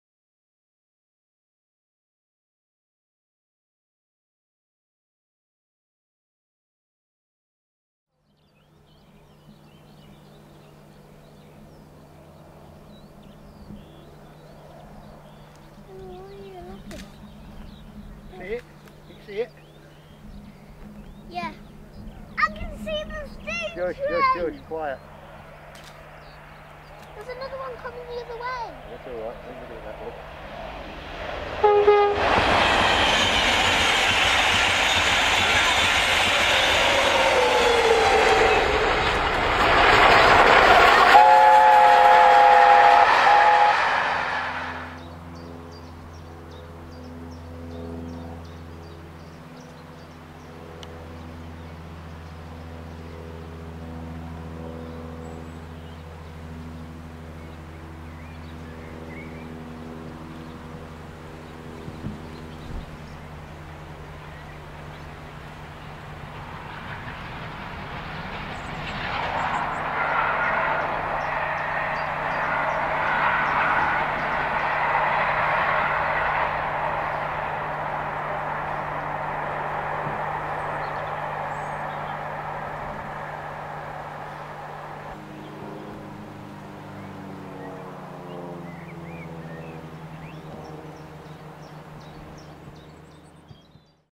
60019 Bittern on The Coronation (Day One) - 15/05/2010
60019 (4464) Bittern on Day One of The Coronation (London Kings Cross - Stevenage - Grantham - York - Morpeth - Berwick - Edinburgh on Saturday 15th May 2010. There is a running commentary and general comments from the father and a neice and a nephew of Mr Bunker the proprietor of Steam Dreams throughout the first clip at Ryther. The second location is at Dalton on the approach to Thirsk. [Sorry? Name drop? Me?]